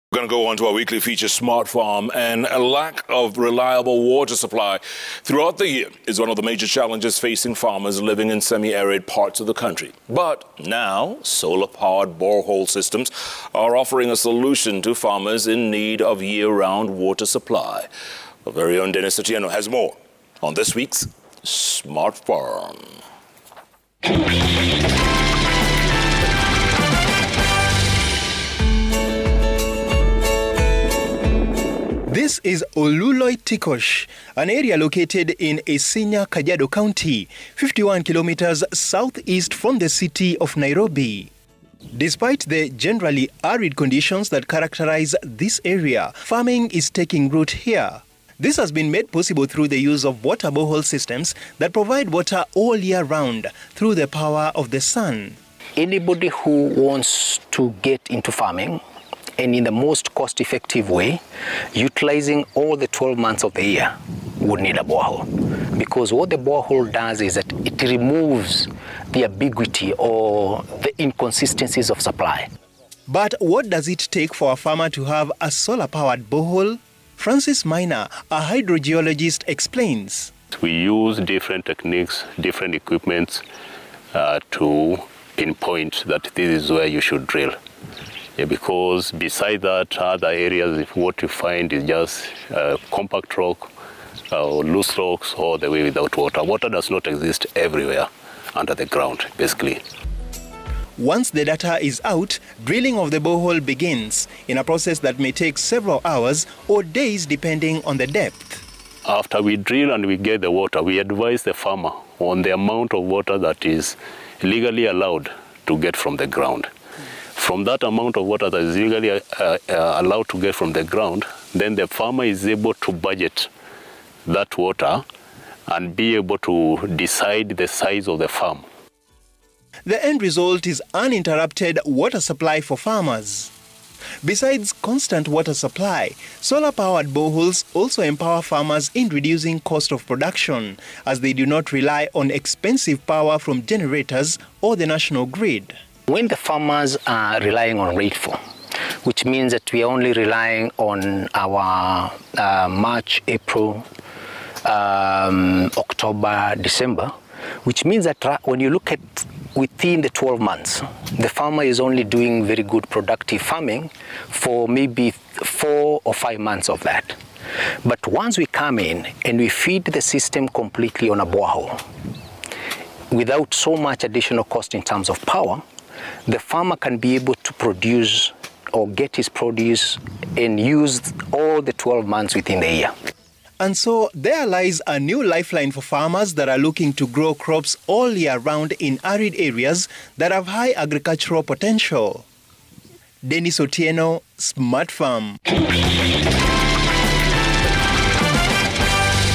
Focus on solar powered farms in Isinya, Kajiado county
Use of borehole systems supporting farming in the dry area